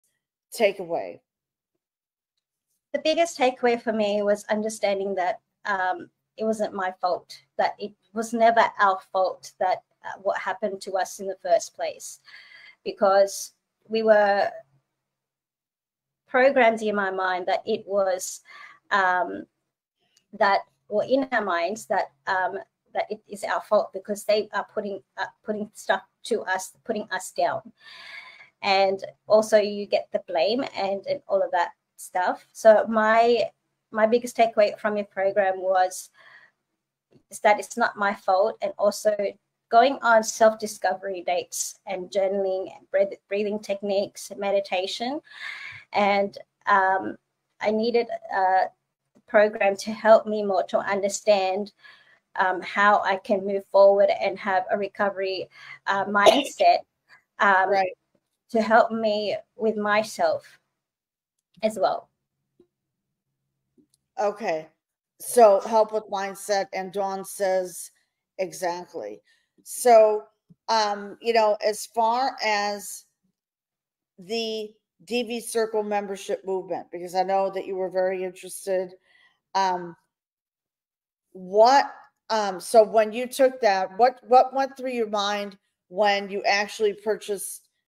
Discovering Self-Love: Transforming Blame into Empowerment
Unlock the path to self-discovery and healing with powerful mindset techniques. Learn how to let go of blame and take control of your own journey towards recovery and self-love. Join us on this transformative journey and embrace a mindset of empowerment.